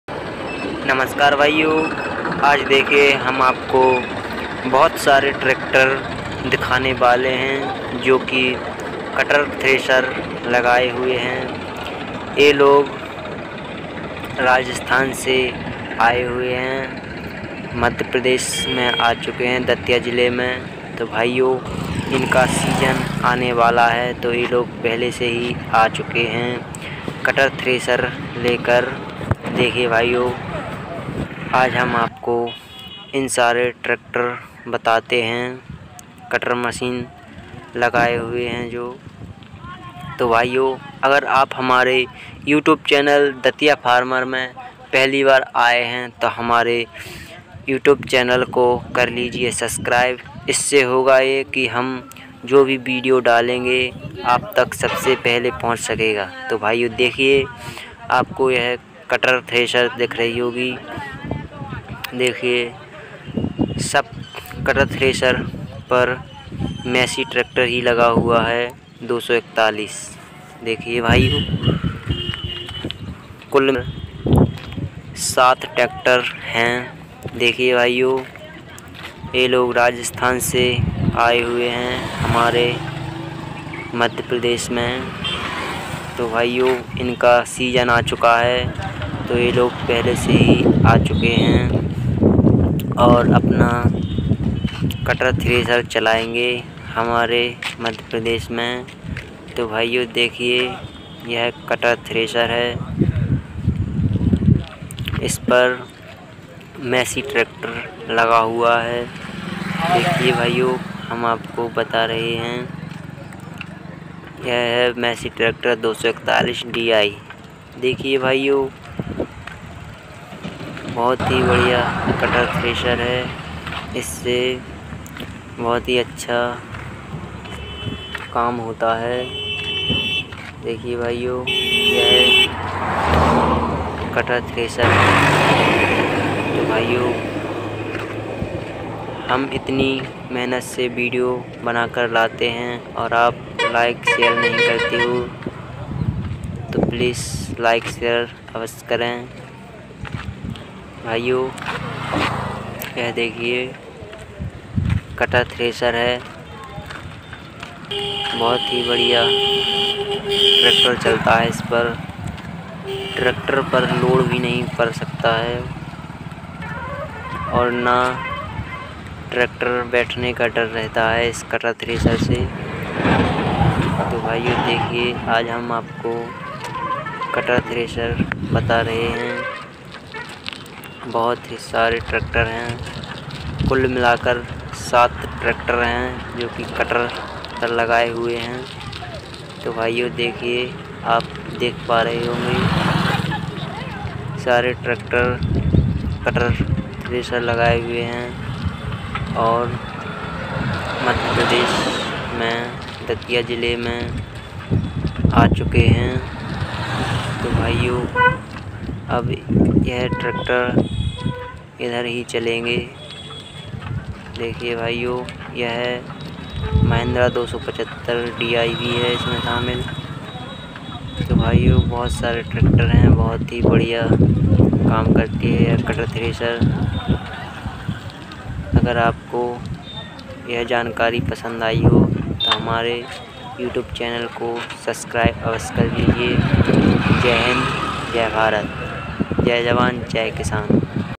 कटर थ्रेशर मशीन एक साथ इतने ट्रैक्टर कभी नहीं देखे होंगे आपने
कटर मशीन थ्रेसर एक साथ इतने ट्रैक्टर आपने कभी नहीं देखोगे इसके बारे में वीडियो बनाया गया है